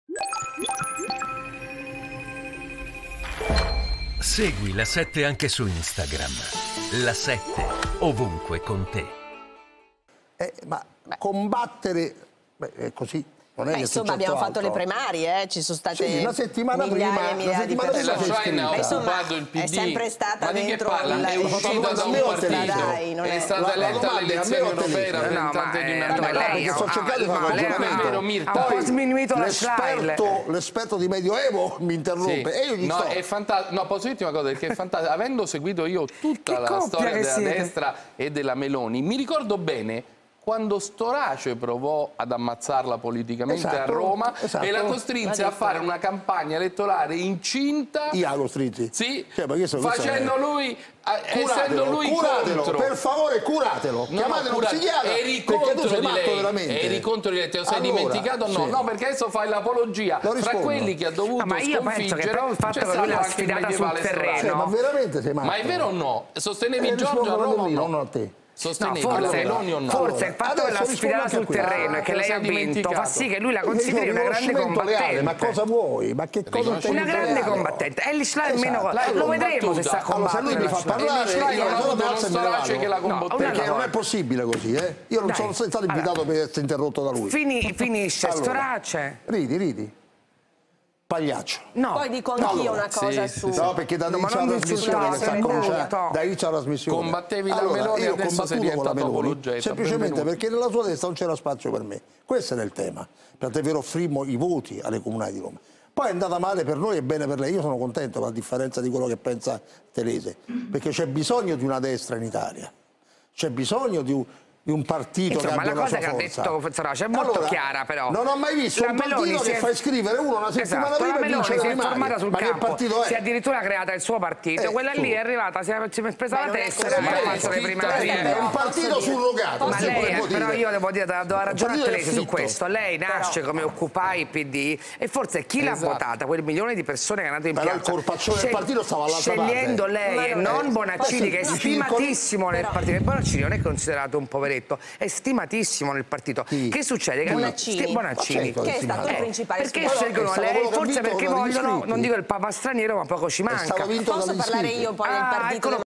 Violento scontro tra Francesco Storace e Luca Telese: "Curatelo, è matto! Pagliaccio! Se non ...
Violento scontro tra Francesco Storace e Luca Telese, che lo interrompe continuamente: "Non è possibile così, io non sono stato invitato per essere interrotto da lui. Se non mi fa parlare mi alzo e me ne vado".